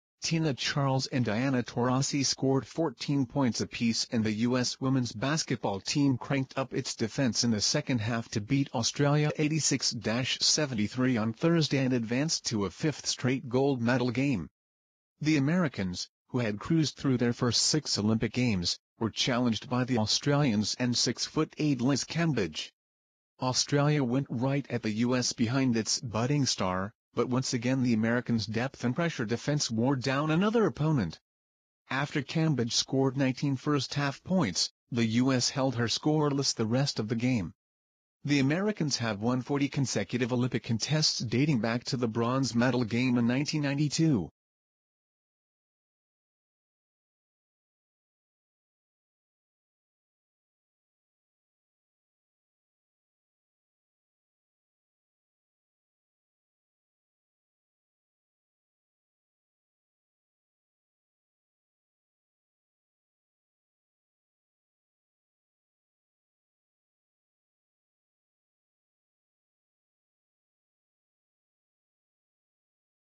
U S Womens Basketball Team Reaches Final
Tina Charles and Diana Taurasi scored 14 points apiece and the U.S. women's basketball team cranked up its defense in the second half to

beat Australia 86-73 on Thursday and advanced to a fifth straight gold medal game.

The Americans, who had cruised through their first six Olympic games, were challenged by the Australians and 6-foot-8 Liz Cambage.

Australia went right at the U.S. behind its budding star, but once again the Americans depth and pressure defense wore down another

opponent.

After Cambage scored 19 first-half points, the U.S. held her scoreless the rest of the game.

The Americans have won 40 consecutive Olympic contests dating back to the bronze medal game in 1992.

Australia and the U.S. had met in the previous three gold medal games. This was the first time they had played in the semifinals since

1996. Australia will play for the bronze medal.